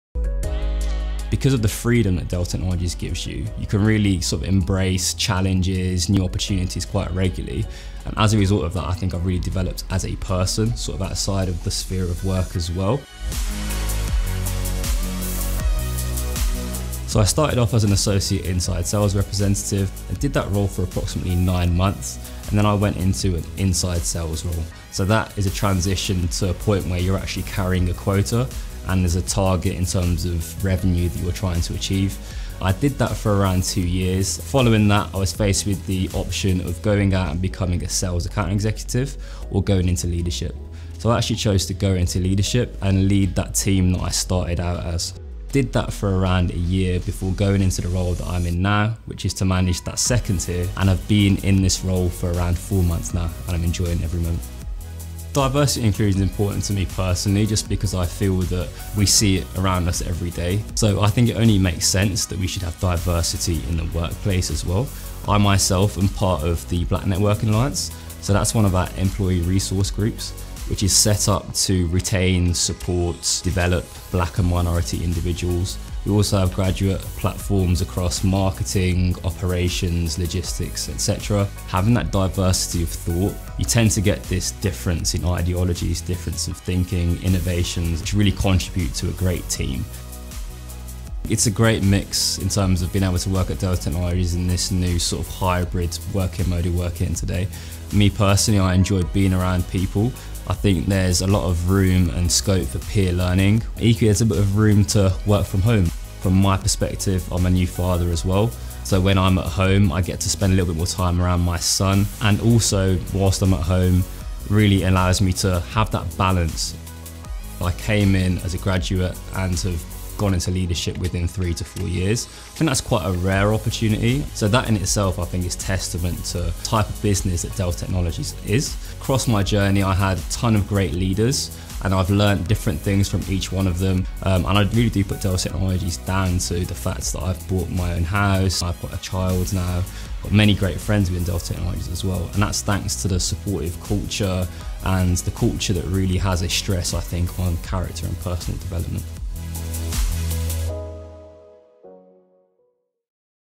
Jamal's Career Story from a Graduate to a Sales Manager
Jamal Downie shares how Dell Technologies contributed to his career journey from starting in the NextGen Sales Academy to becoming a Sales Manager.